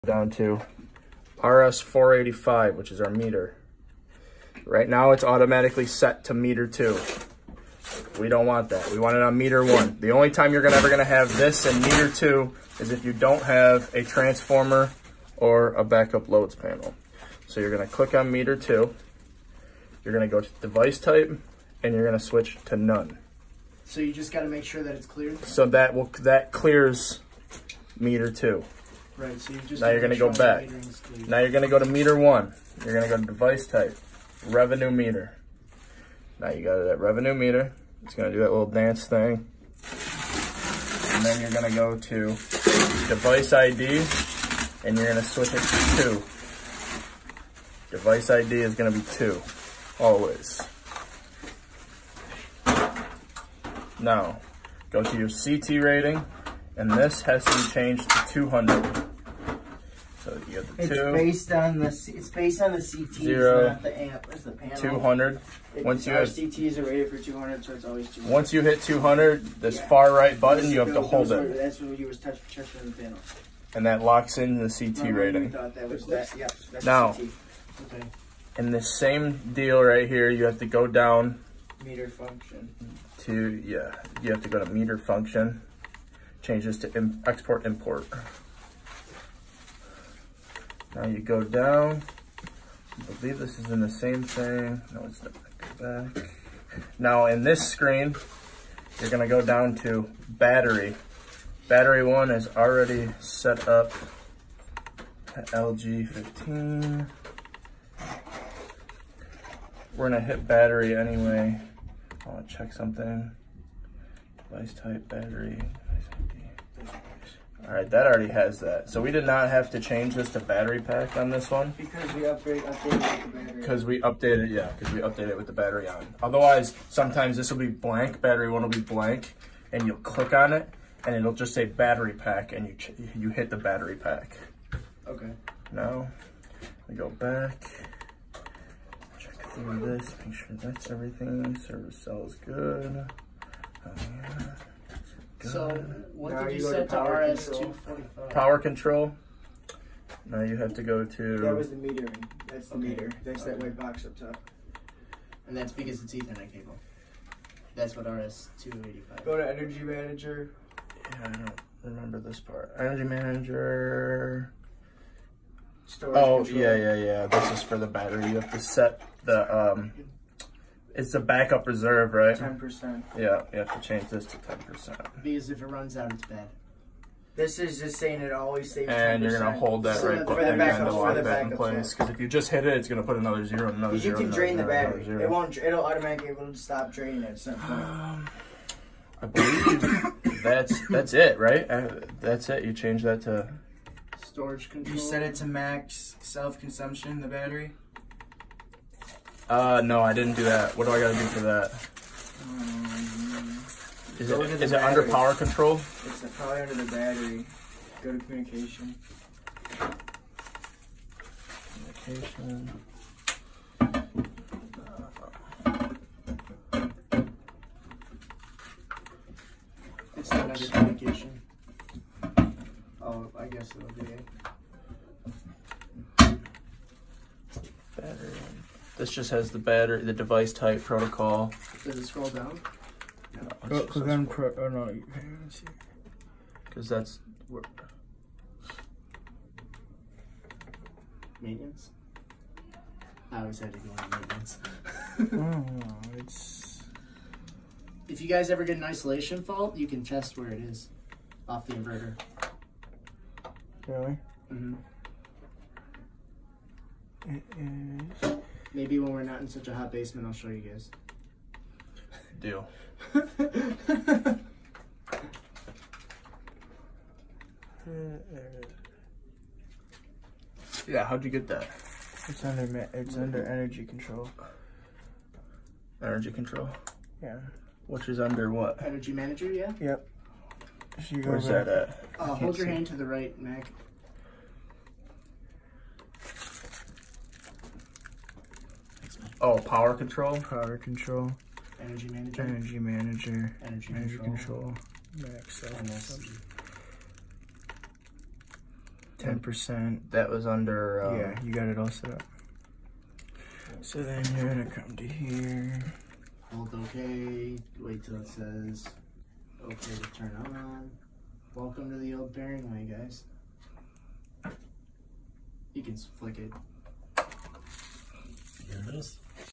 LG chem battery Commission. Solar edge inverter
How to commission solar edge inverter with LG chem battery.